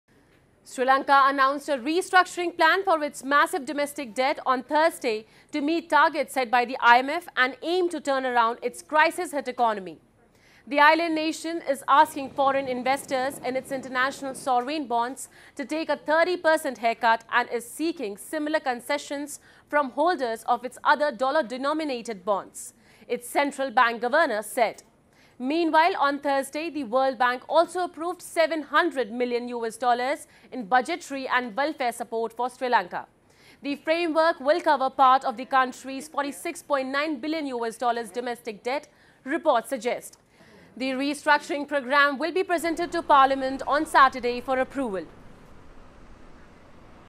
Sri Lanka asks dollar debt holders for 30% haircut
Sri Lanka announced a restructuring plan for its massive domestic debt on Thursday to meet targets set by the IMF and aim to turn around its crisis-hit economy. The island nation is asking foreign investors in its international sovereign bonds to take a 30% haircut and is seeking similar concessions from holders of its other dollar-denominated bonds, its central bank governor said. Meanwhile on Thursday, the World Bank also approved $700 million in budgetary and welfare support for Sri Lanka. The framework will cover part of the country's $46.9 billion domestic debt, reports suggest. The restructuring programme will be presented to parliament on Saturday for approval.